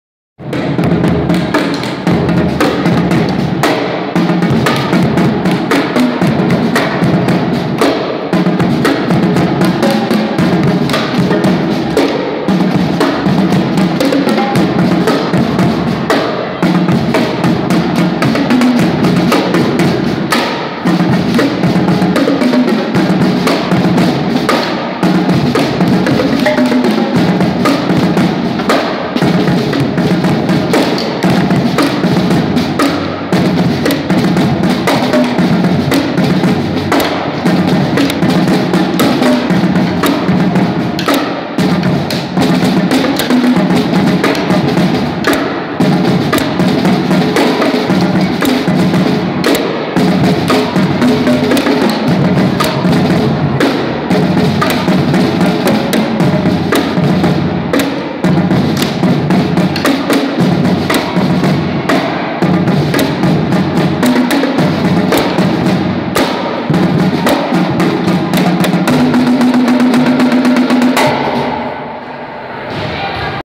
Drum Corps.
Us playing at the volleyball game